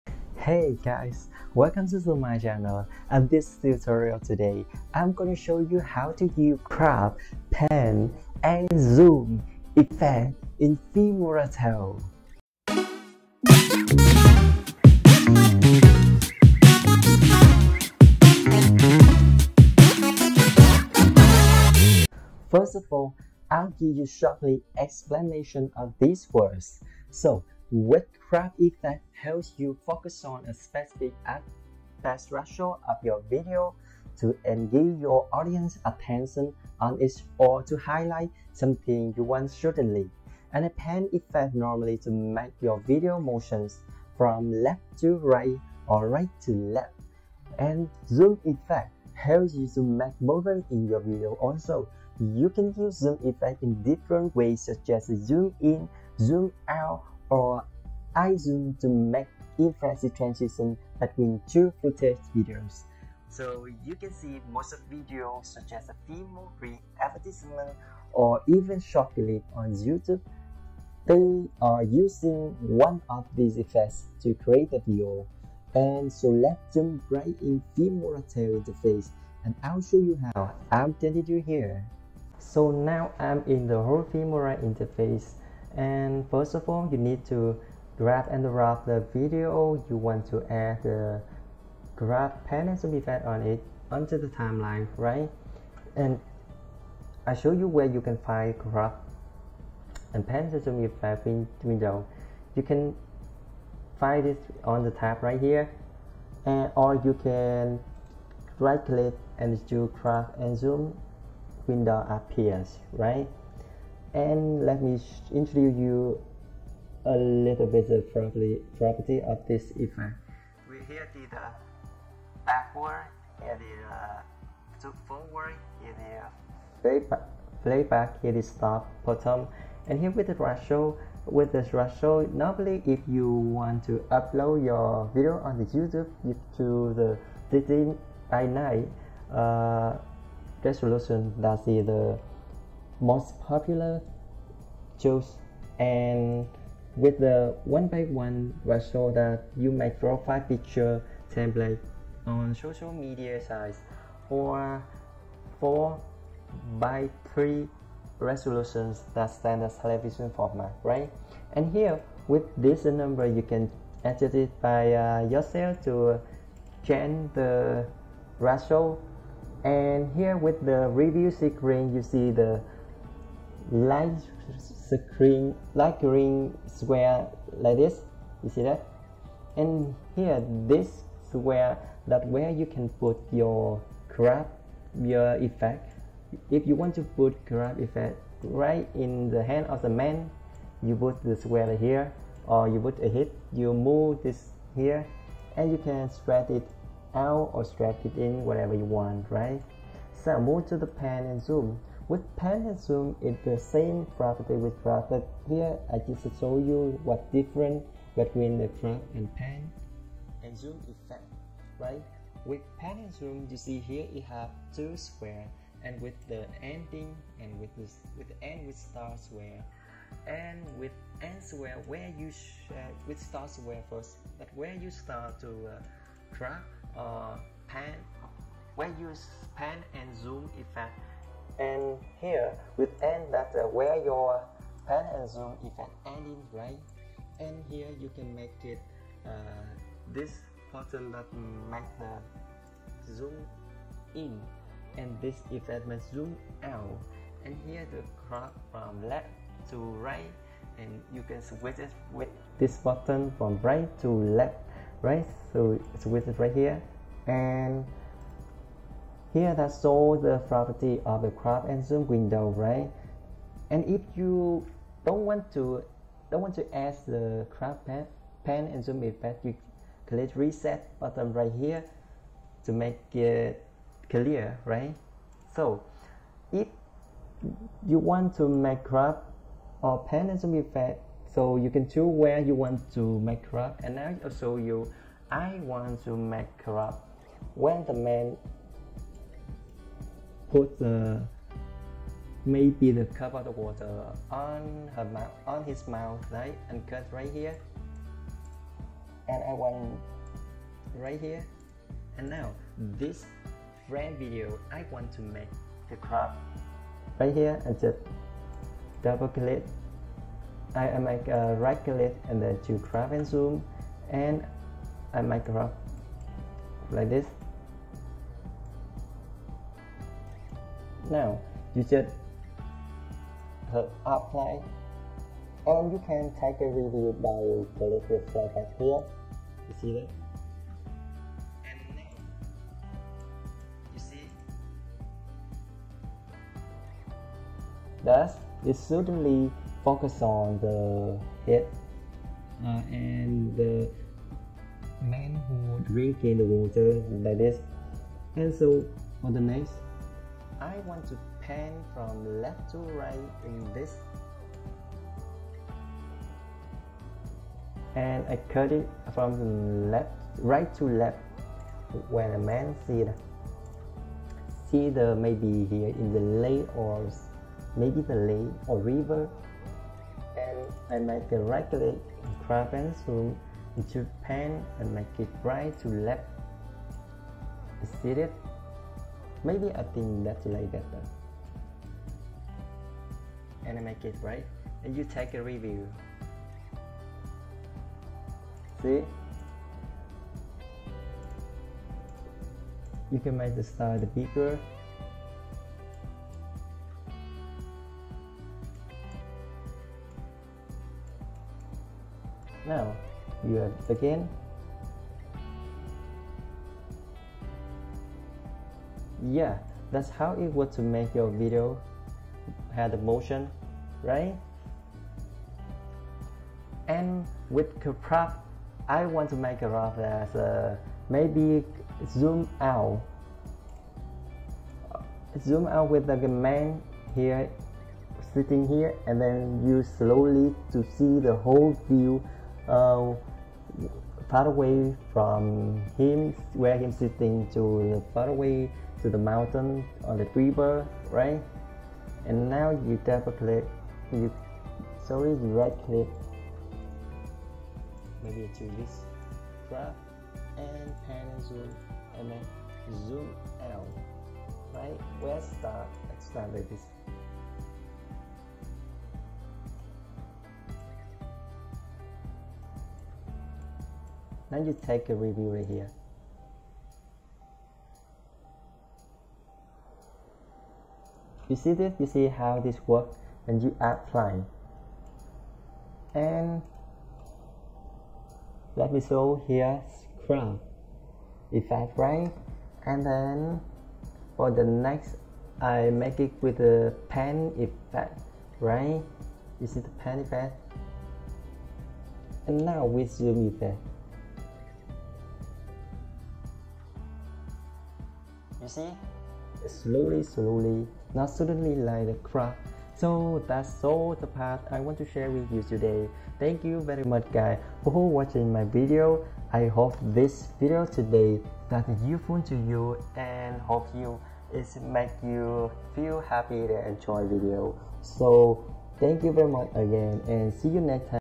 HOW TO USE CROP, PAN AND ZOOM IN FILMORA 12
This tutorial today shows you how to use Crop, pan and zoom effect in Filmora 12 with basic steps for begginers.
Here, All things that I tried my best, hope that you Enjoyed This Video :)

✅Click the 🔔 to stay updated on the latest uploads! 👍 Thumbs Up if you like this video. ❤️Thank you! ❤️